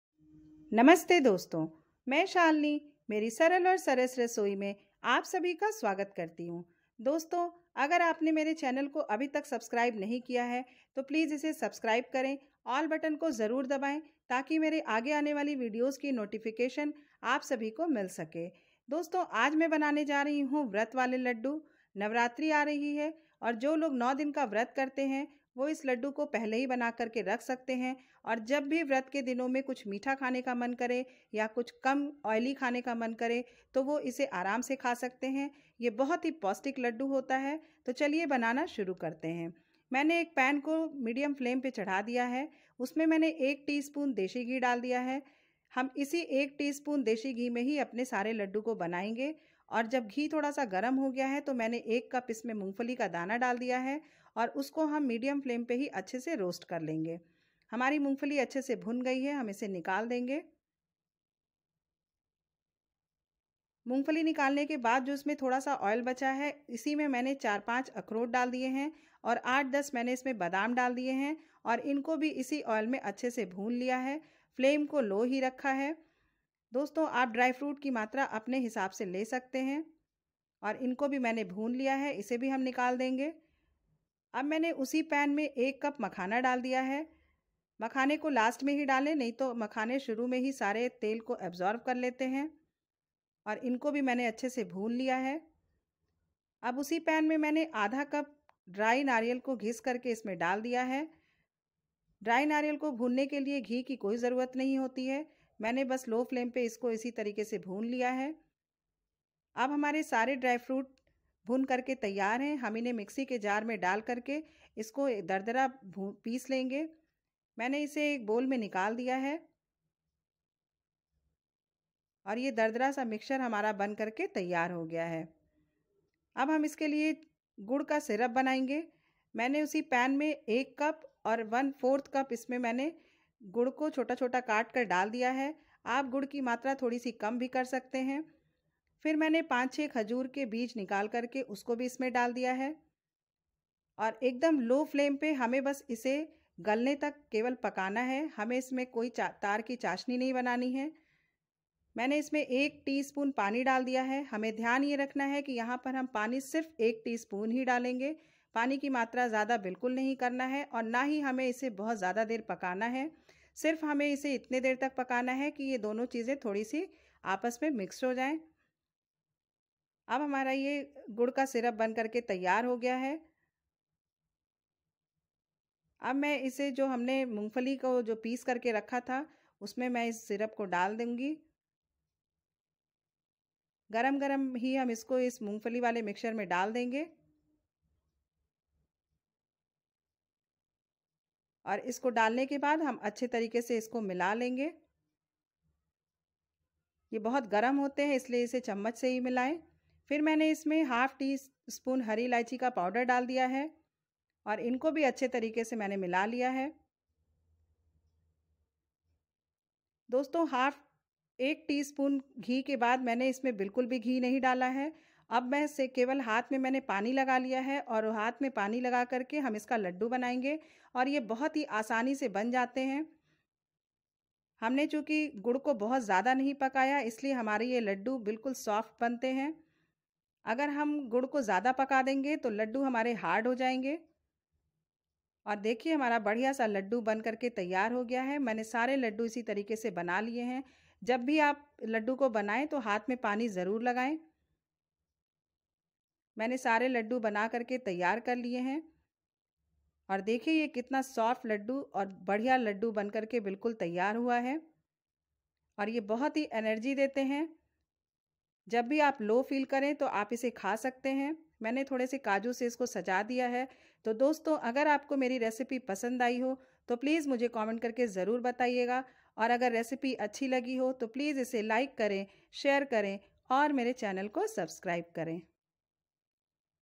नवरात्रि में बनाएं एक चम्मच घी से पौष्टिक लड्डू । Vrat Ke Ladoo
Aaj meine banaya h vrat k ladoo jo ki moong fali aur dryfruits ko milaker bana h. ye 1 ts ghee mein hi acche se ban jaatein h.Mujhe umeed h ki ye energy full ladoo aap sub ko pasund aaenge.

Ingrediants-
desi ghee-1 ts
moongfali-1cup
walnut-4-5
almond-7-8
makhana-1cup
crush dry nariyal-1/2cup
gud-1-1/4cup
khajoor-6-7
water-1ts
hari elaichi-powder-1/2ts

Other links-
YouTube URL-ww.youtube.com/saralaursarasrasoi